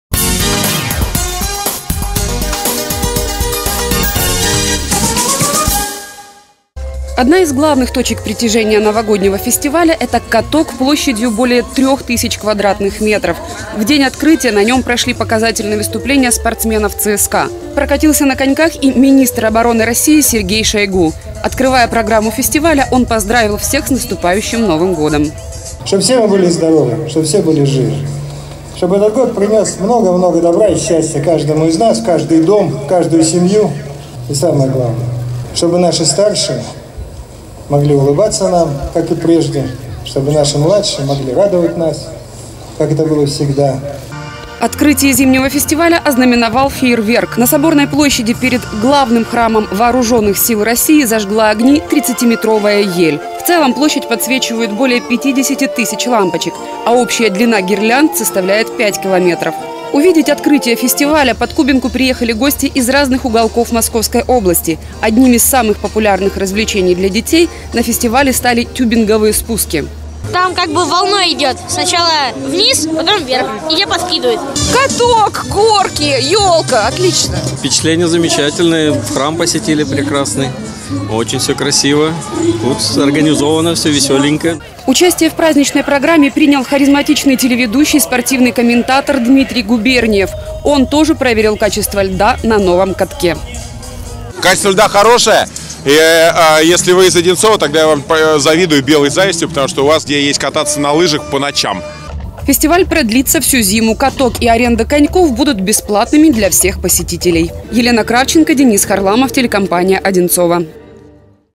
Новогодний фестиваль
Ярмарка, 30-метровая новогодняя елка, каток, тюнинговые спуски. На Соборной площади Главного храма Вооруженных Сил России открылась крупнейшая новогодняя площадка Подмосковья. Гостей фестиваля приветствовал  министр обороны России Сергей Шойгу.